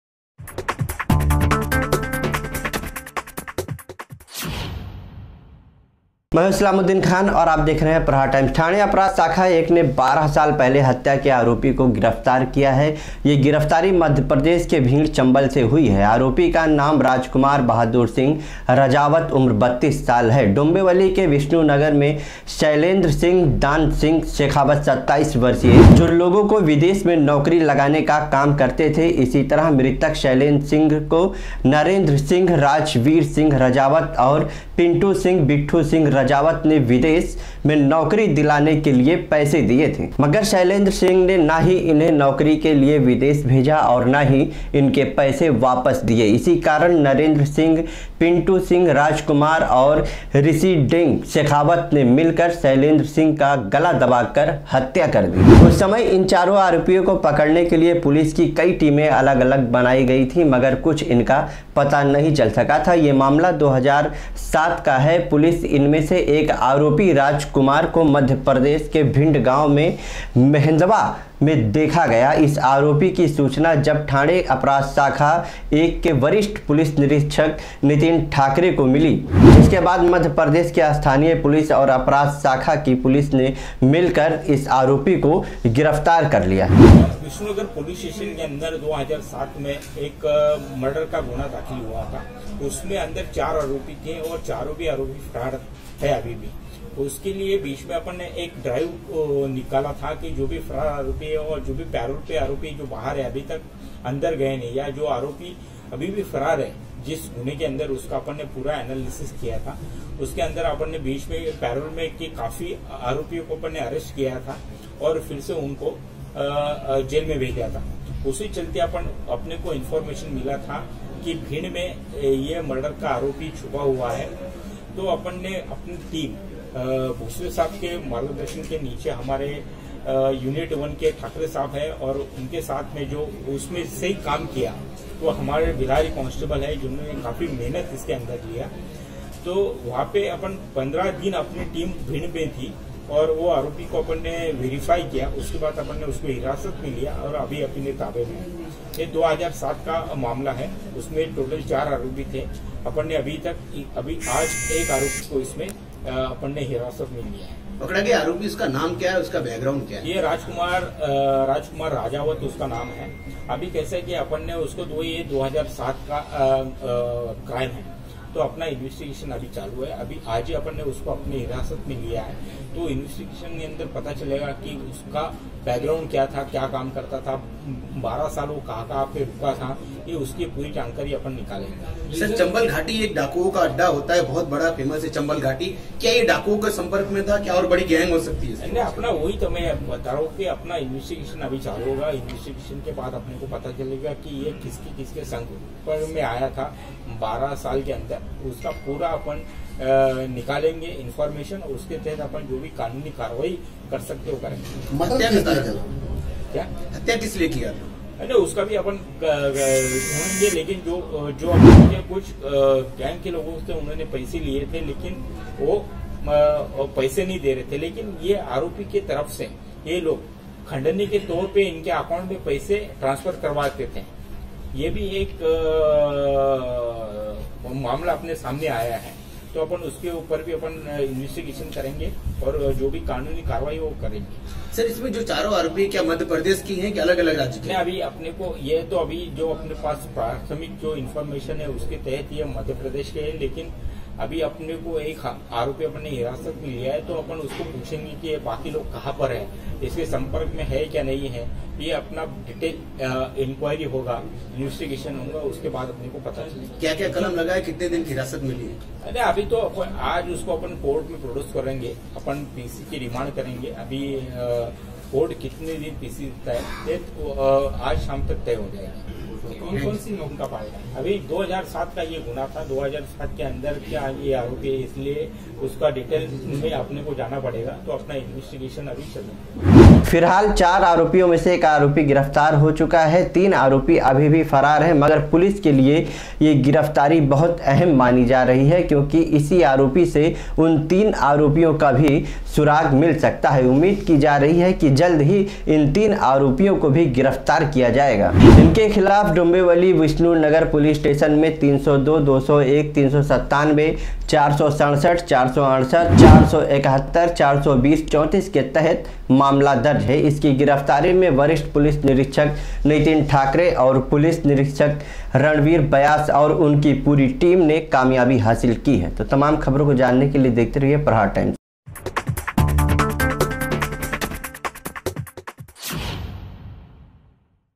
बारह साल पहले हत्त्या का आरोपी चम्बल से गिरफ्तार ,Prahar Times -
बारह साल पहले हत्त्या
का आरोपी चम्बल से गिरफ्तार

डोम्बिवली में हत्या कर चार लोग फरार हो गए थे

मध्यप्रदेश के चम्बल से अपराध शाखा ने किया गिरफ्तार

ठाणे अपराध शाखा  एक की बड़ी कामयाबी मानी जा रही है

prahar times